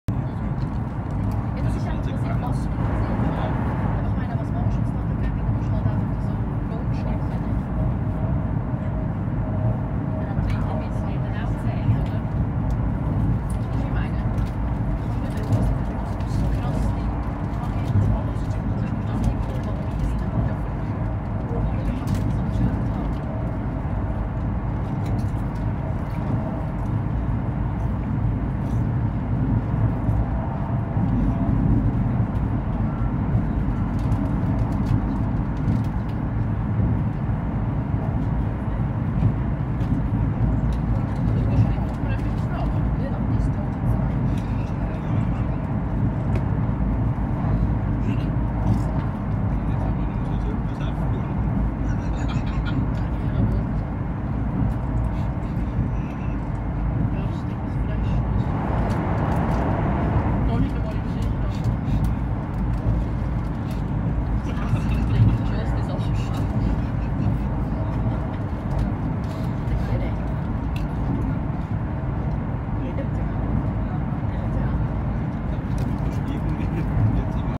One minute on the Hong Kong - Guilin High Speed Train
A snapshot of the journey on the Hong Kong - Guilin High Speed Train. This train travels at a maximum speed of 300km per hour, but stays below 200km per hour in Hong Kong.
This is an unedited sequence of footage to capture a moment in time. No edits.